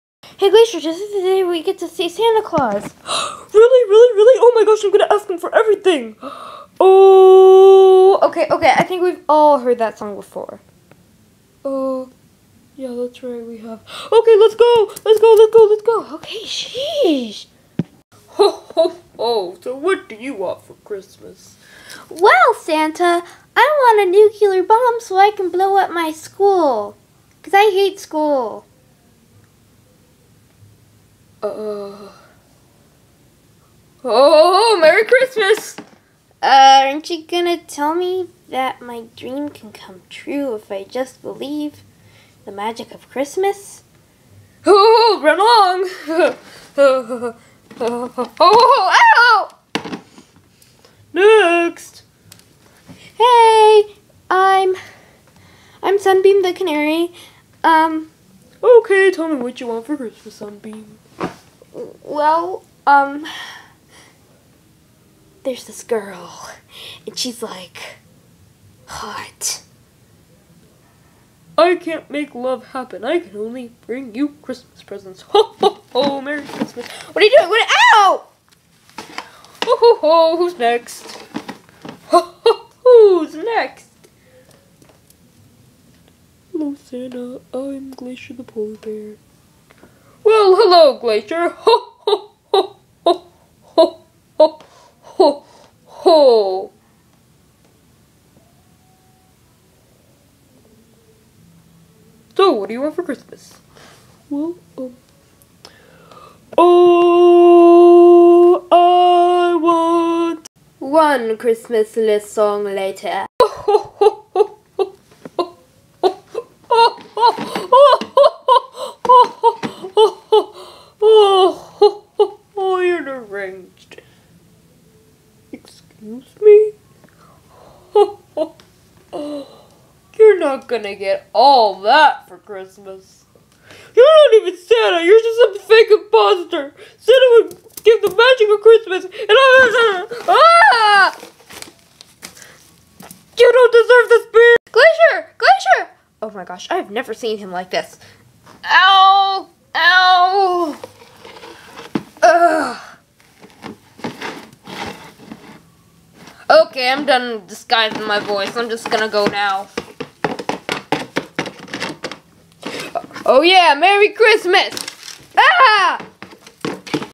Glacier and Blizzard go see "Santa"
Well there ya have it! xD. Glacier goes to see the mall "Santa" and tells him what he wants and well. Just watch! xD
God Bless! :3
~koiboykinz8~